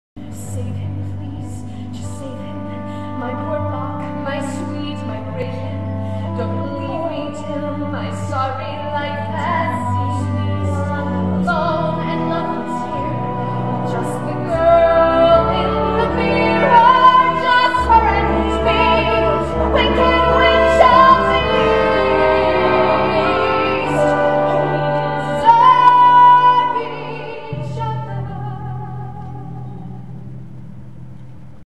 Going to See Wicked
Going to See wicked June 3rd 2008

Cast: Stephanie J. Block (Elphaba), Kendra Kassebaum (Glinda), David Burnham (Fiyero), Jayne Houdyshell (Madame Morrible), Lenny Wolpe (The Wizard), Cristy Candler (Nessarose), Ben Liebert (Boq), Steven Skybell (Doctor Dillamond)